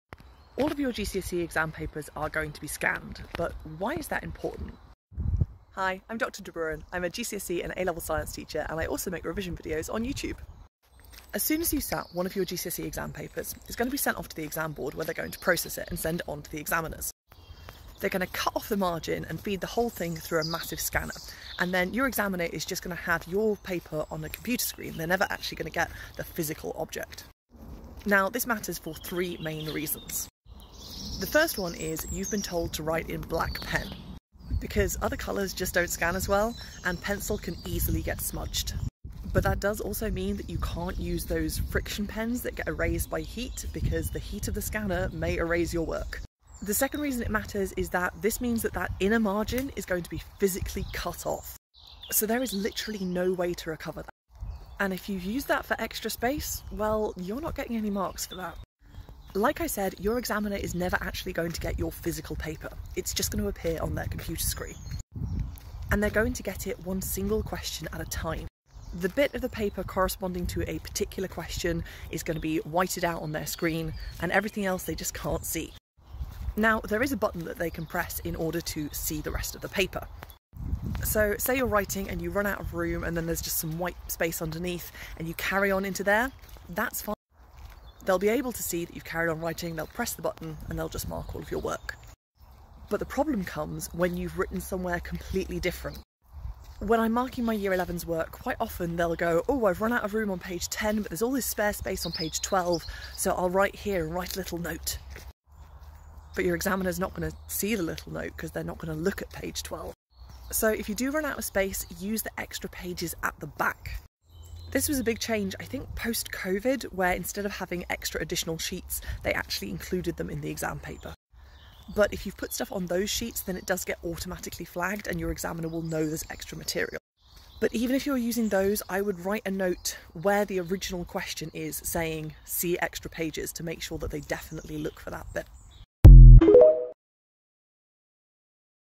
What happens to your exam paper after the exam is over?
Your exam paper is going to be scanned, but why is that important?